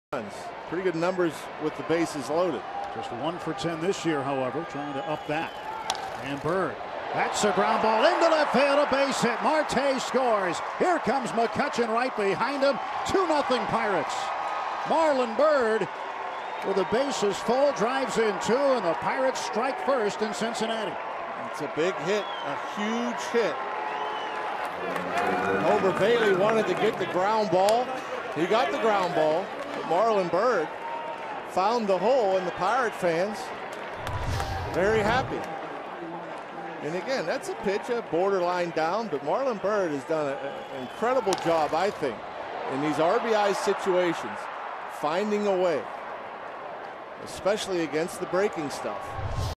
PIT@CIN: Byrd's base hit scores a pair in the third
9/27/13: Marlon Byrd grounds a single through the hole to bring around Starling Marte and Andrew McCutchen for a 2-0 Pirates lead